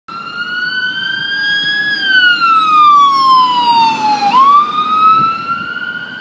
Princeton, Illinois Fire Department Brush 871 Responding to possible fire
Units dispatched
Princeton medic 842
Princeton brush 871
Princeton police